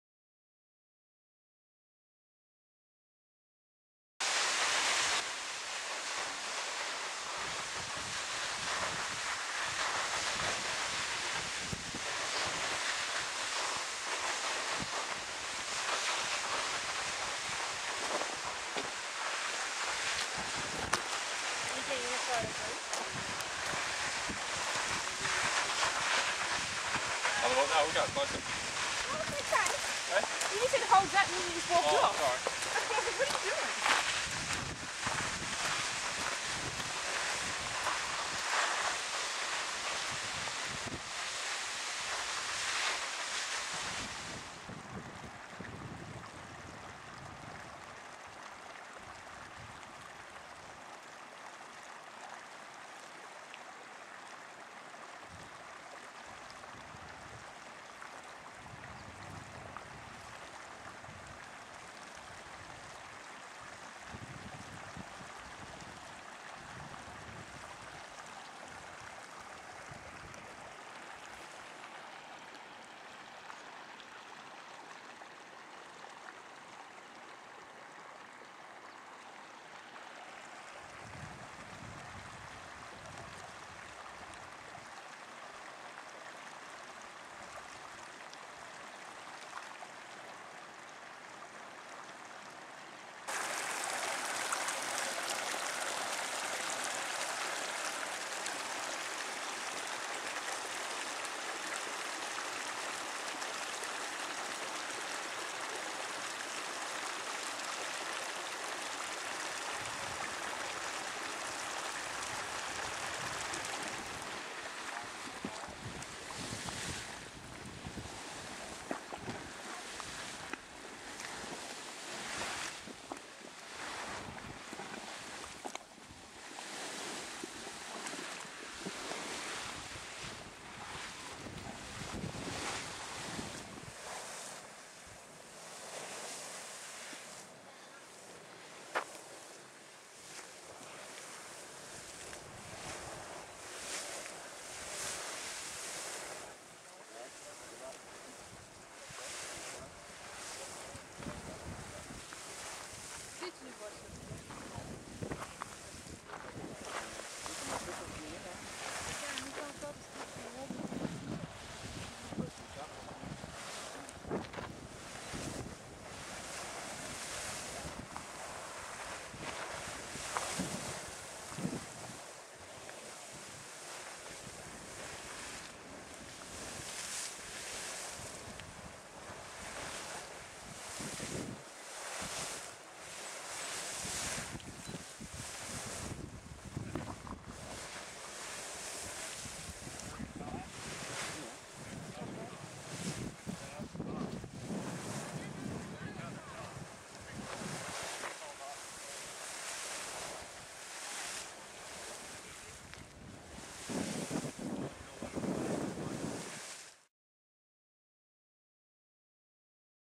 Imperial Geyser & Fairy Falls
This video is from a hike my dad and I made out to a back country geyser named Imperial Geyser.  The trail to the geyser also leads past a pretty waterfall.  I remember once hiking out to this geyser in 1985 when I was a kid.  At that time, the geyser didn't reach more than 6 feet in height.  25 years later, the geyser was much more vigorous, with some bursts reaching at least 30 feet.